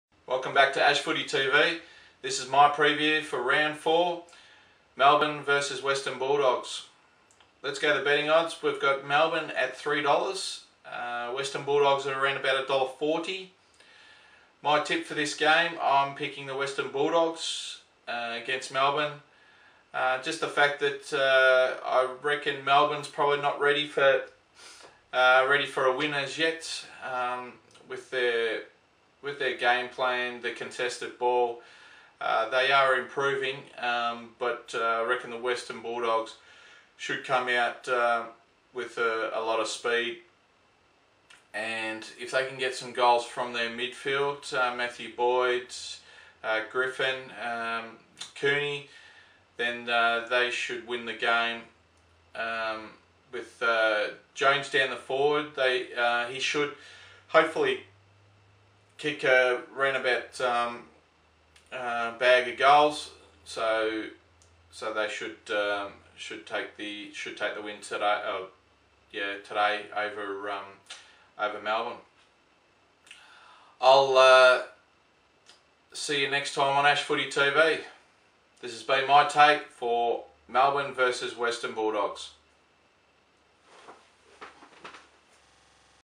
AFL 2012 - Round 4 - Game 9- Preview - Melbourne vs Western Bulldogs
Ash Stevens gives you a preview of the Round 4 - Game 9 - Melbourne vs Western Bulldogs Plus gives his predictions.